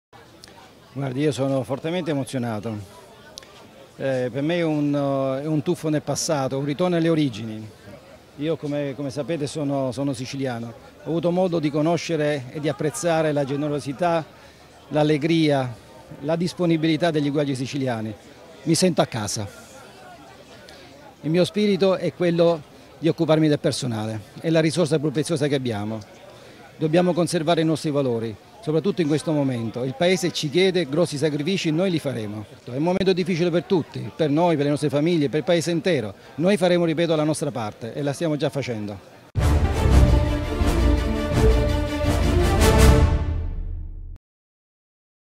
AUGUSTA - INTERVISTA AMMIRAGLIO MARIO CULCASI
Breve intervista al nuovo comandante di Comforpat Contrammiraglio Mario Culcasi.